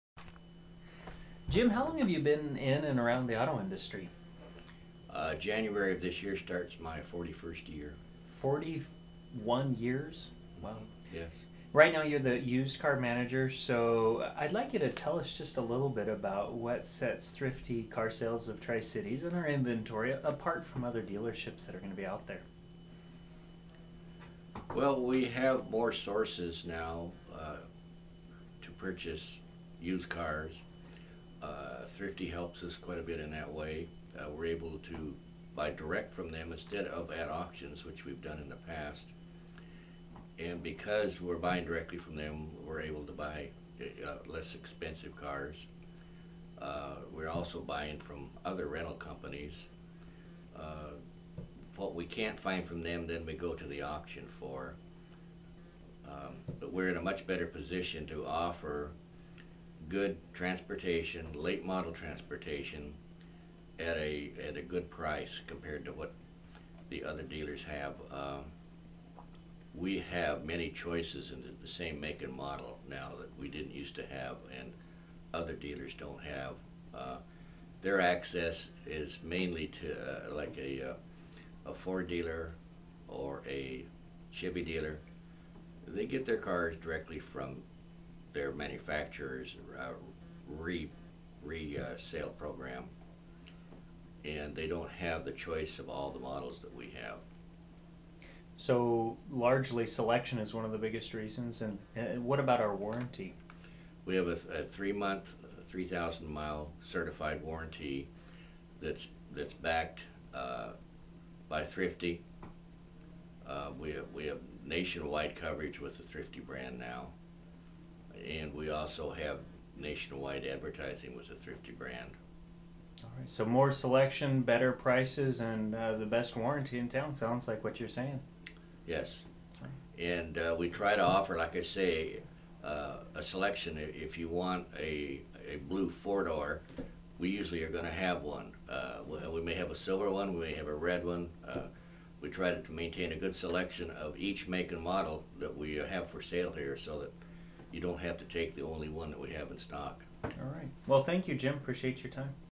Jim Smith drives home why you should buy Thrifty
Jim Smith is the used car manager at Thrifty Car Sales of Tri-Cities. He brings 40 years of buying experience to Thrifty. Being a Thrifty Car Sales Dealership means we have direct access to the national Thrifty Car Sales inventory.